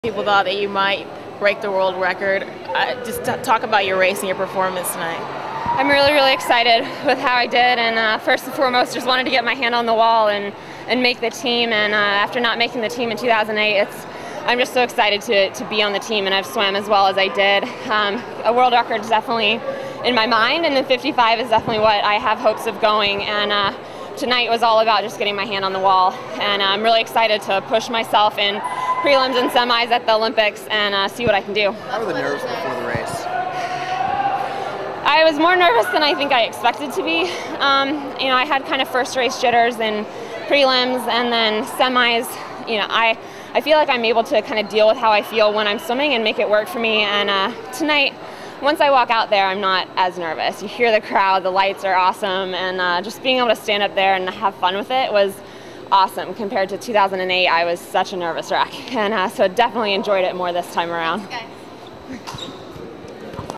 Dana Vollmer
Dana Vollmer discusses her 100 meter butterfly on night two of trials.